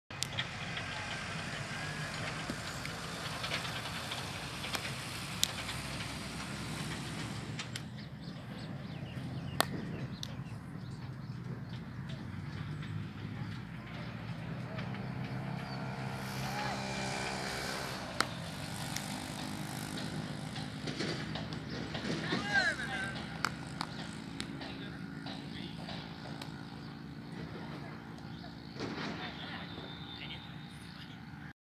Slip Catching Practice
This is a great way to get the kids a feel of the onfield catching in slips